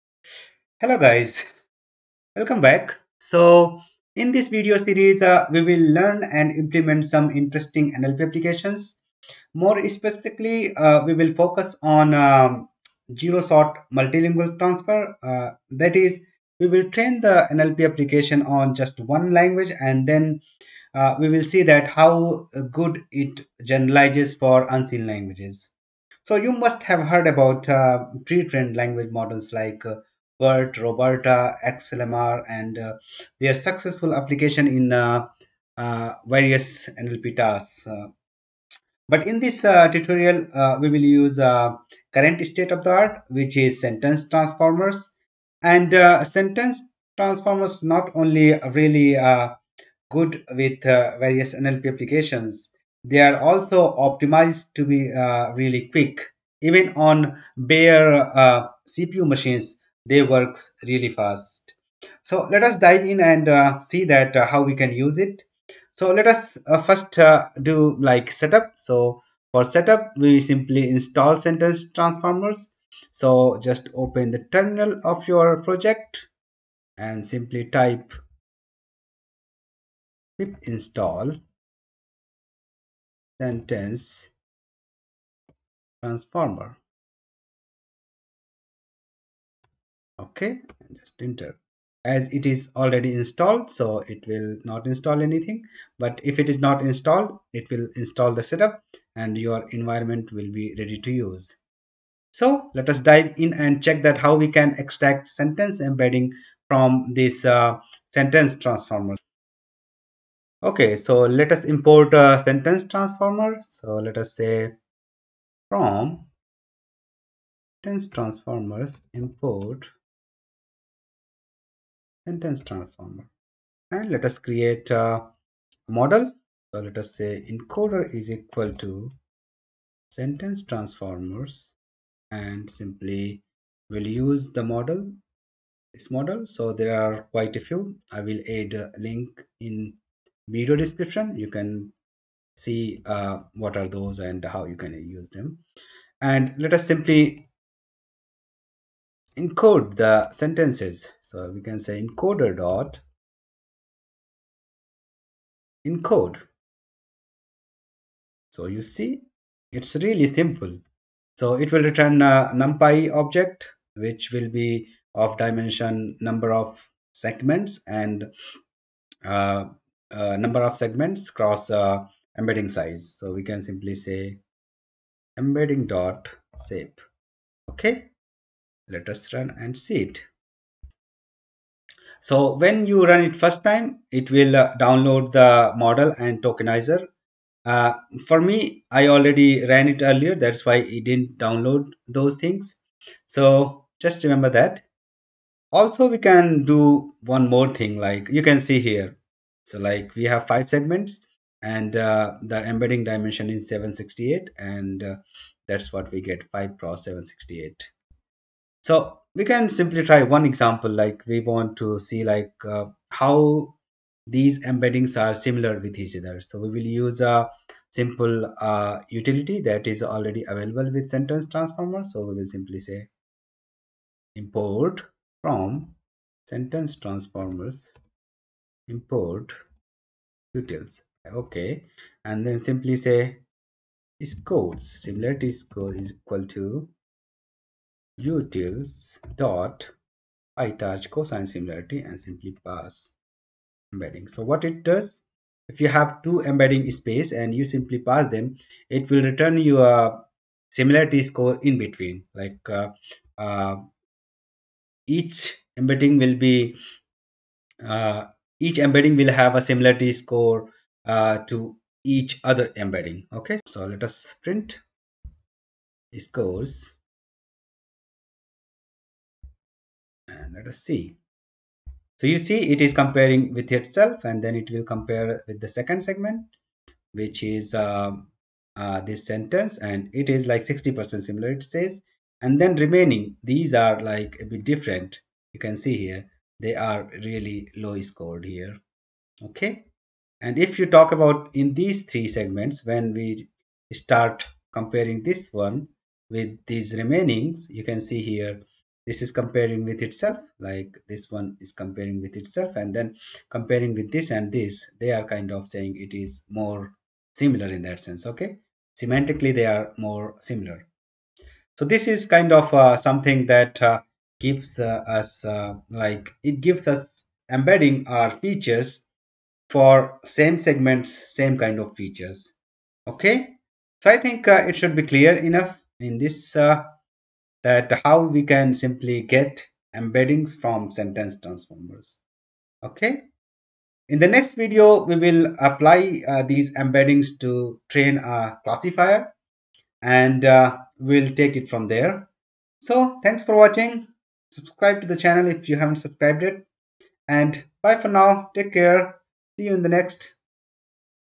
Multilingual NLP: #1 | Compute Embeddings from Pretrained Language Models
Multilingual NLP: Compute Embeddings from Pretrained Language Models

With this video, we'll start a series on Multilingual NLP. In this series, we'll discuss and implement some basic NLP applications having zero-shot multilingual capability. More specifically, we'll use pretrained language models to achieve the multilingual transfer.

Papers-
Sentence Transformers-

Chapters:
0:00 - Introduction
1:06 - Setup
1:41 - Compute embeddings
3:28 - Text Similarity
5:35 - Next